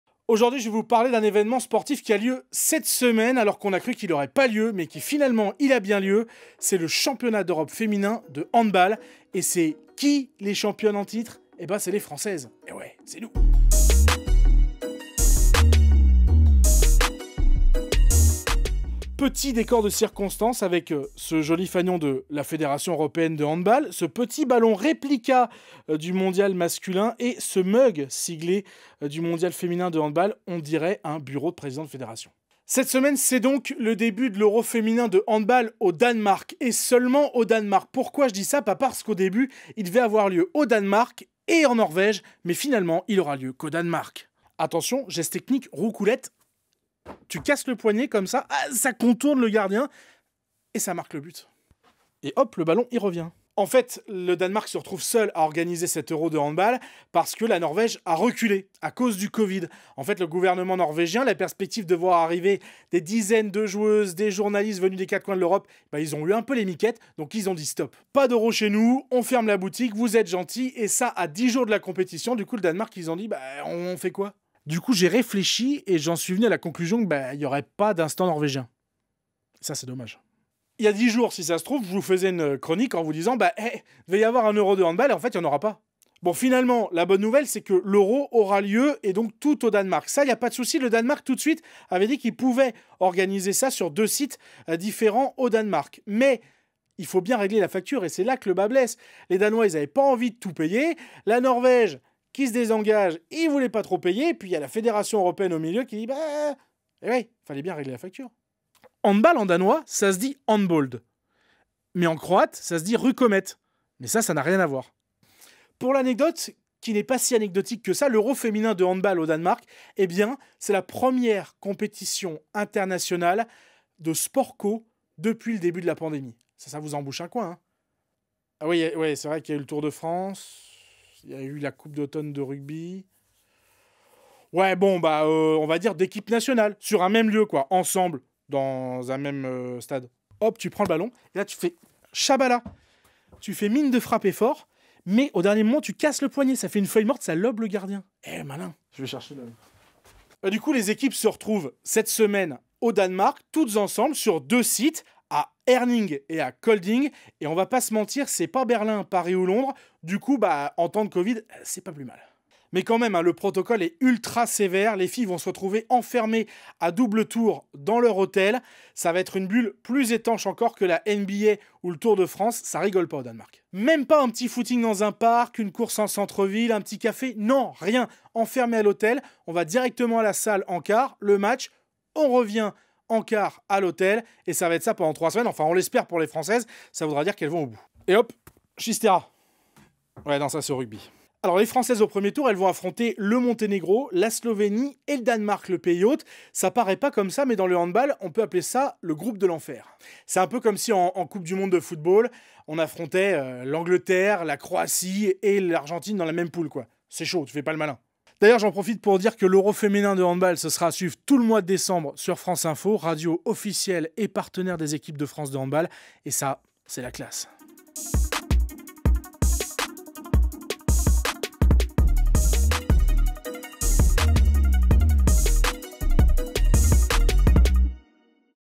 L'Euro de handball féminin aura bien lieu, mais uniquement au Danemark
Cette semaine, c'est le début de l'Euro de handball féminin qui aura lieu seulement au Danemark. La gouvernement norvégien a décidé de se retirer de l'organisation à cause du Covid-19. On vous explique.